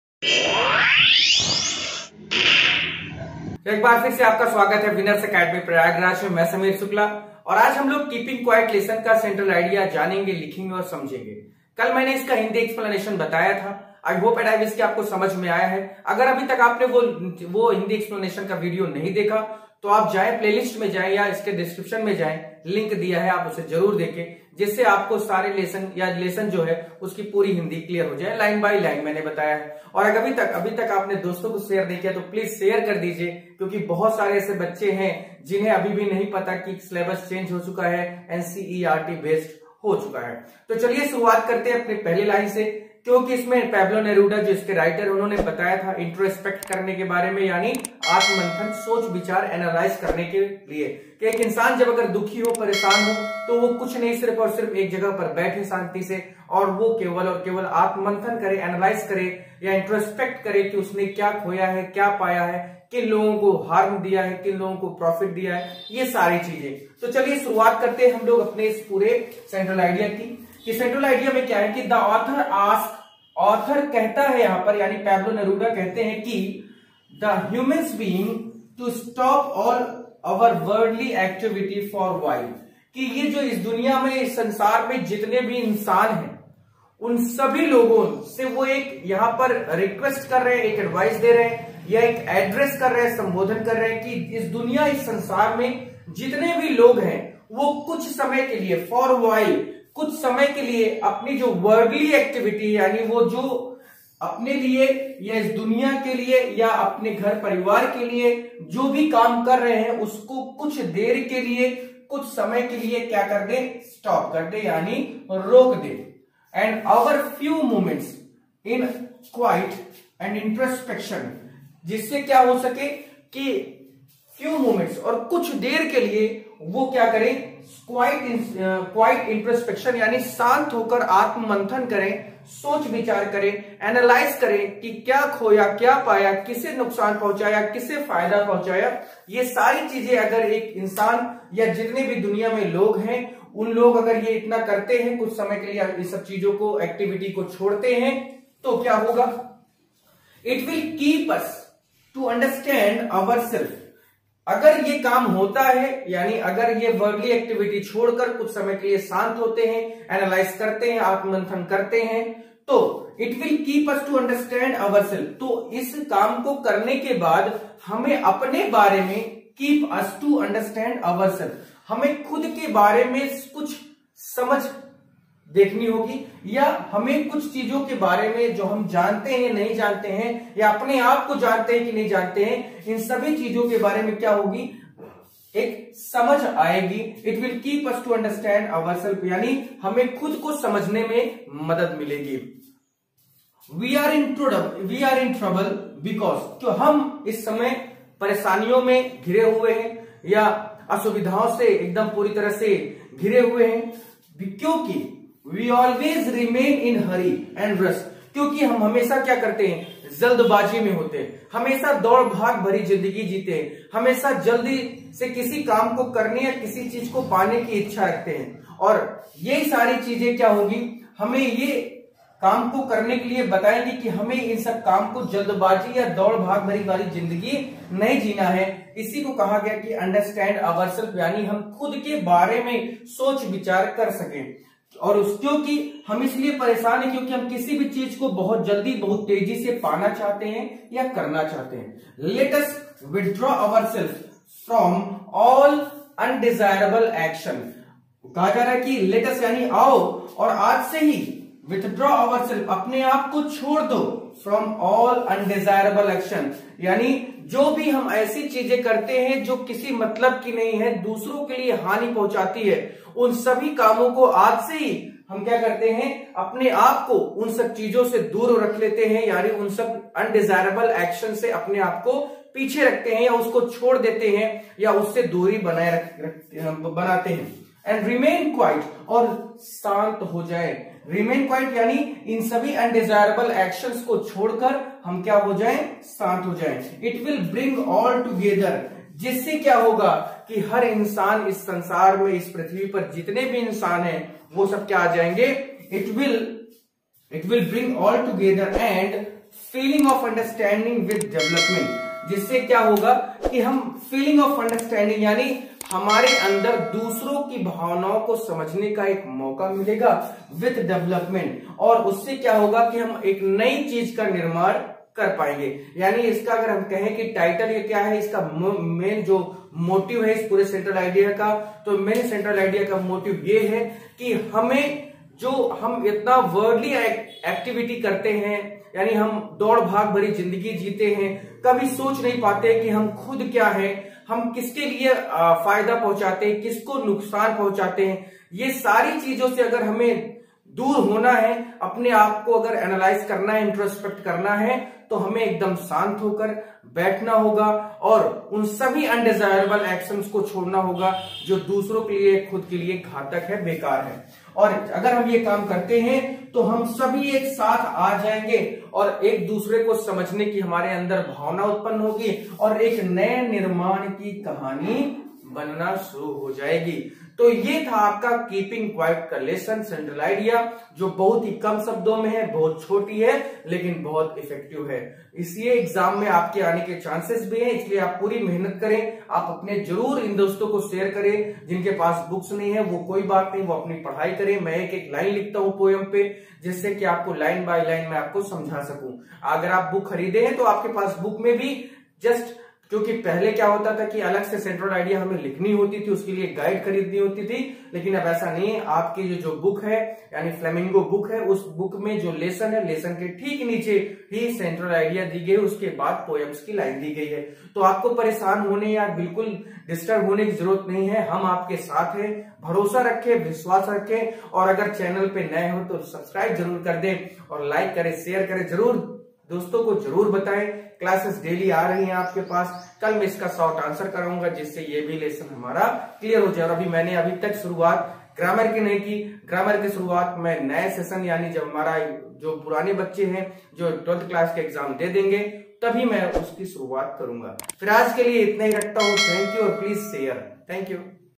keeping quiet || Central idea || Up Board || Class 12 || By Sameer Sir
in this video we have provided best video of verb from syntax in a very simple way that how to wrote attractive and impressive  and with trick for up board  upcoming board exams so watch complete video till last